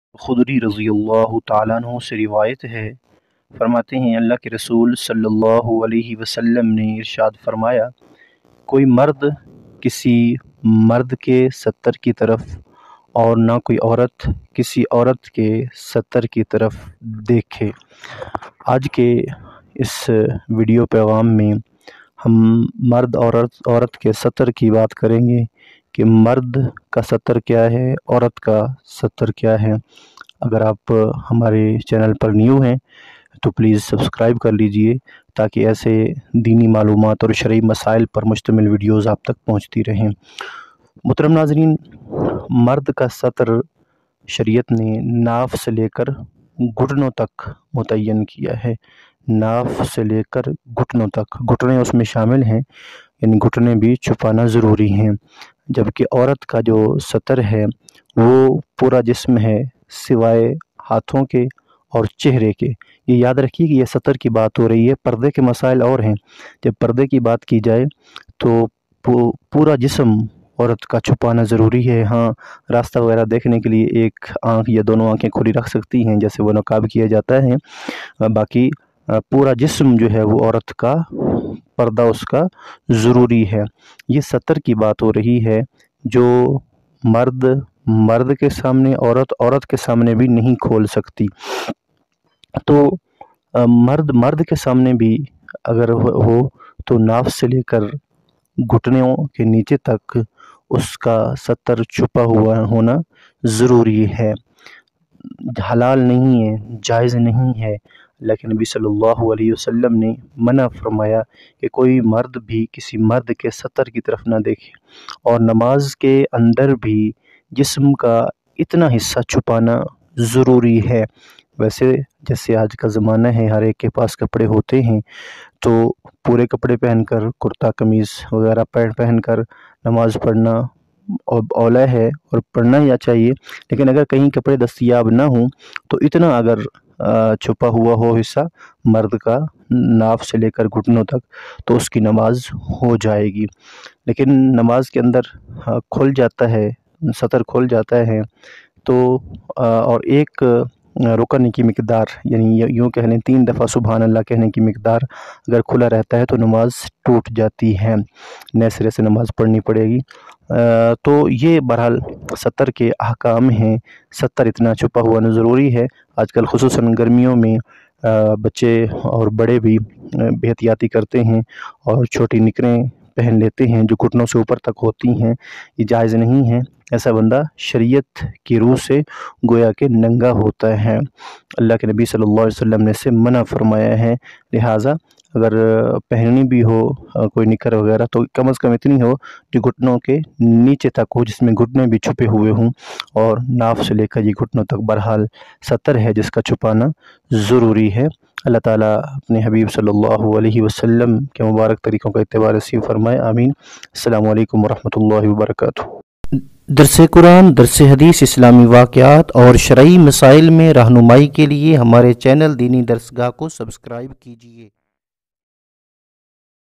مردو عورت کاستر یعنی جسم کاوہ حصہ جس کاچھپاناضروری ہے | Hiden parts of human body according to islam
عورت اور مرد کے ستر کوتفصیلا اس ویڈیو میں بیان کیا گیاہے۔